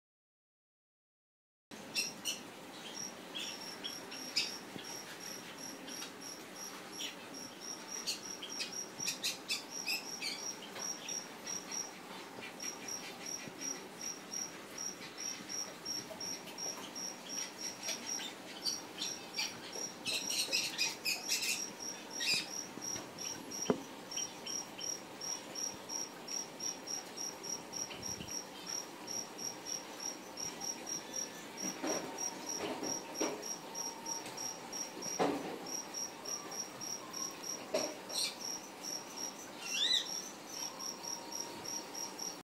ぺちゃんこハムスター Crushed funny hamster
よくこんな体勢で寝れますね。ぺたんとしています。おもしろい顔が潰れてるけど可愛い。キュートな寝相の赤ちゃんハムスター。

Can he sleep in such a posture well? It is lovely although the face is crushed. The baby hamster of a cute sleeping posture.Animal_Movie